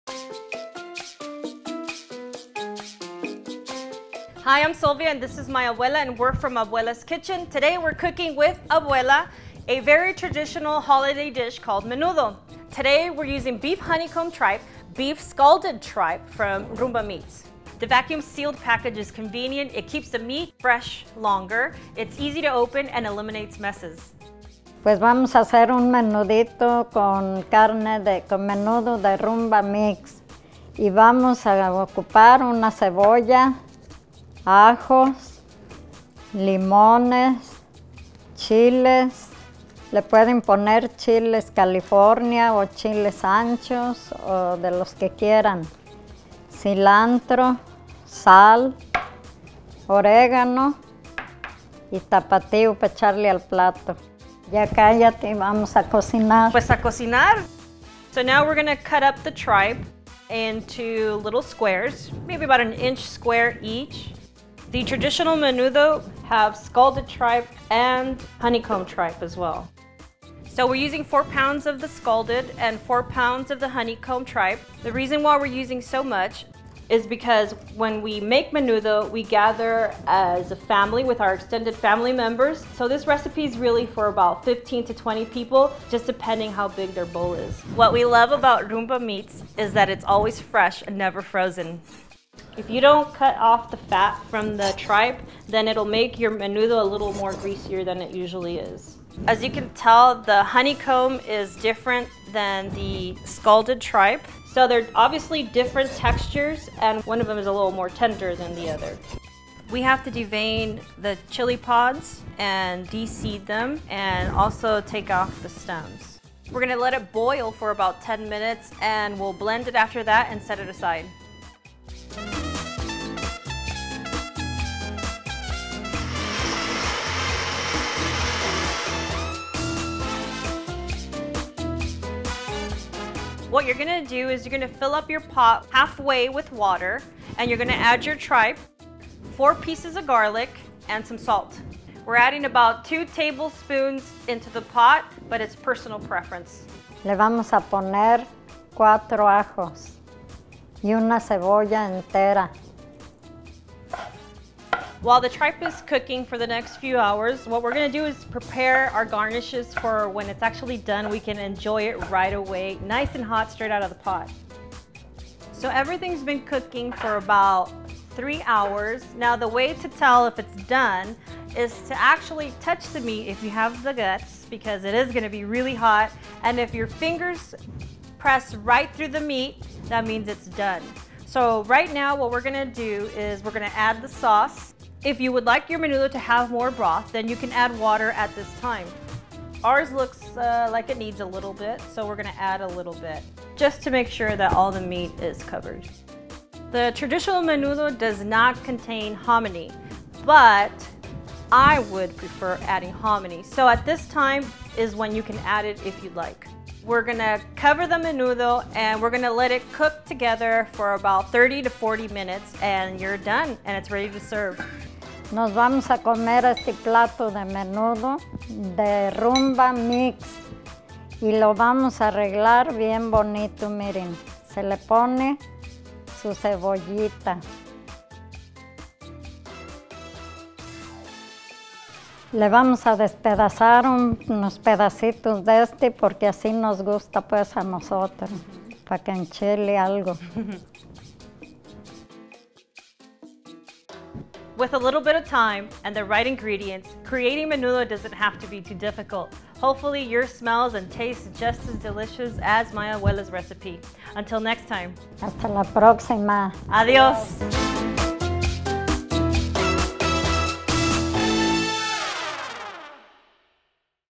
Menudo recipe: Abuela's Kitchen cooks with Rumba Meats
Ready for some authentic Mexican cooking with Silvia and her abuela? Watch them prepare a favorite holiday dish, a menudo recipe featuring scalded and honeycomb tripe. Visit rumbameats.com for more information.